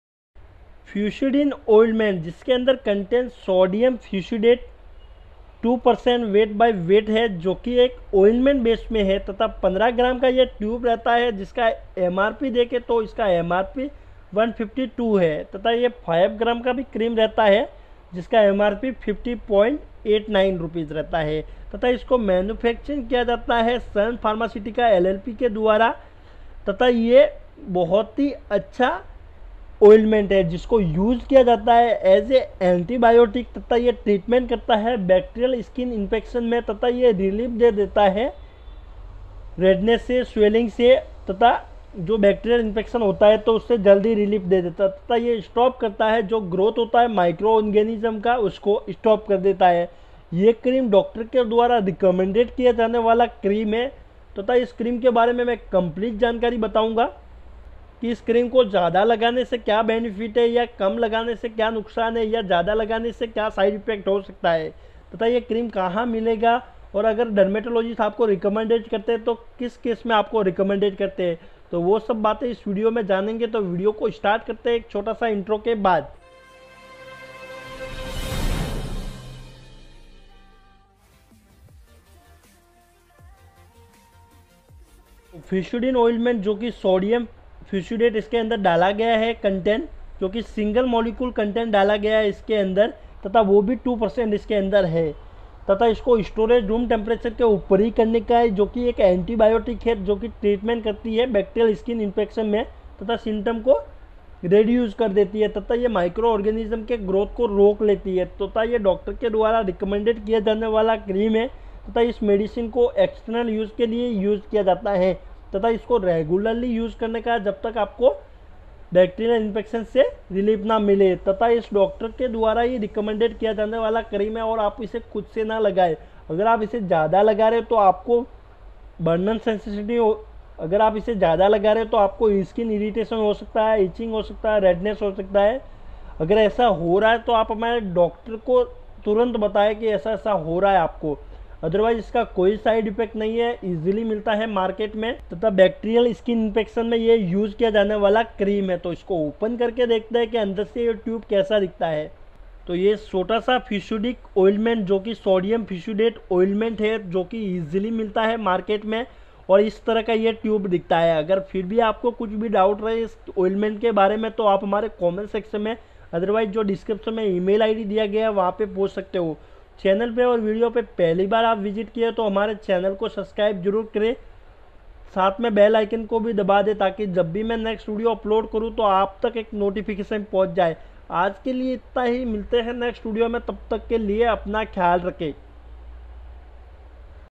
Fucidin Ointment, Sodium fusidate ointment, Fucidin Ointment uses side effects and benefits review
Fucidin Ointment, Sodium fusidate ointment, Fucidin Ointment uses side effects and benefits review in hindi  by @Medicineknowledgehindi

kindly note